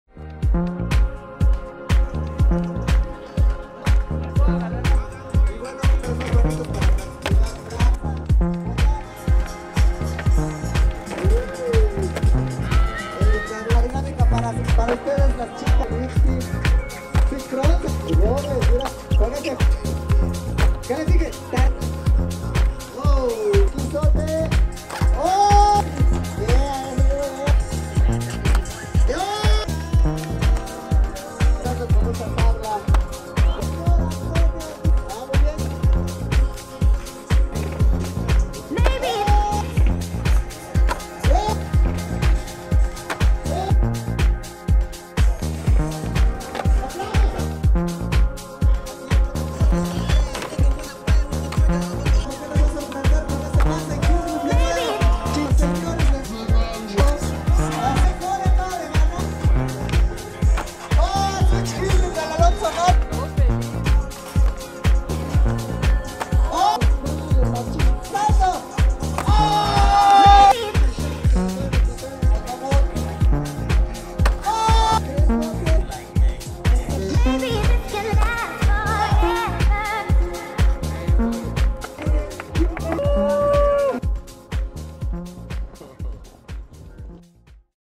Adidas skateboarding
Este 20 de Julio en todo el mundo se celebro el pro model de Ty Shawn J. con adidas skateboarding y en CdMx se organizo el KEEPMEXCLEAN para invitar a la gente a patinar en el circuito de botes de basura y ganar dinerillo mientras nos divertiamos.